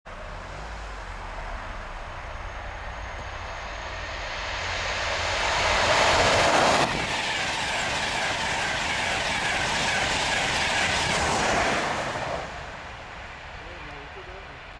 Class 91 Musings from Botany bay One more 91!.mpg
...and just one more time another 91 heading south no toot this time, appearing out of the gloom of British (nearly) springtime. (C) RCT 2011